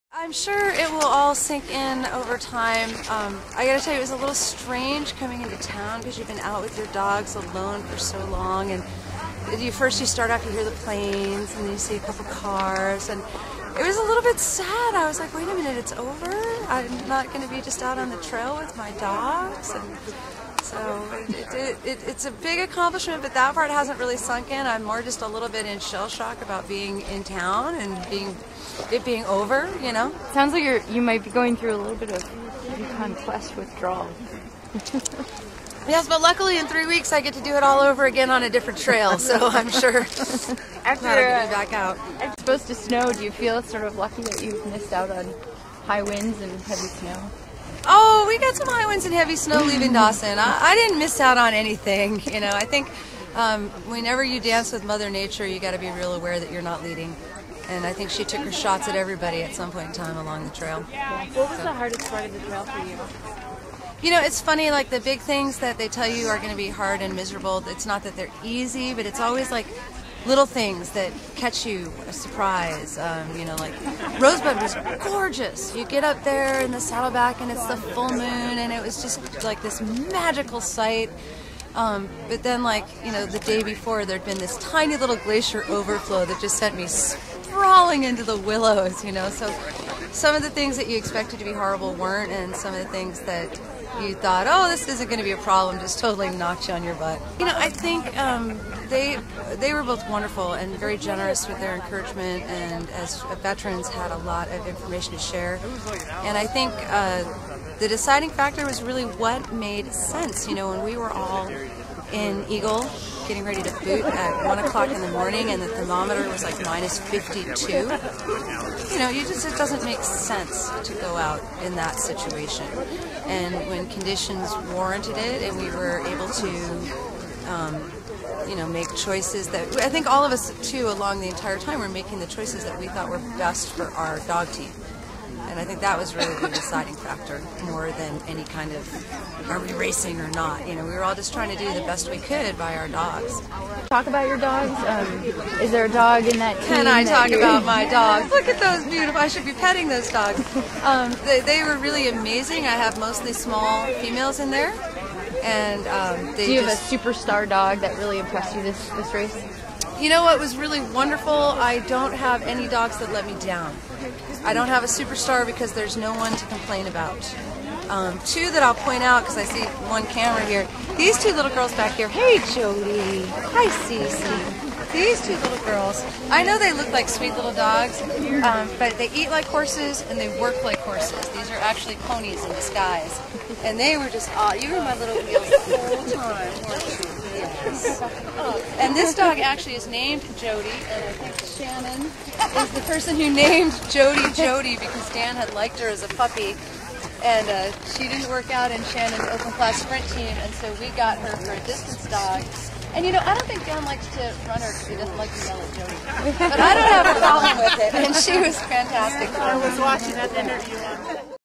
YQ 2011: Jodi Bailey finish line interview
Jodi Bailey talks about the race at the finish, complimenting her dogs and trailmates David Dalton and Mike Ellis.

Video: John Perreault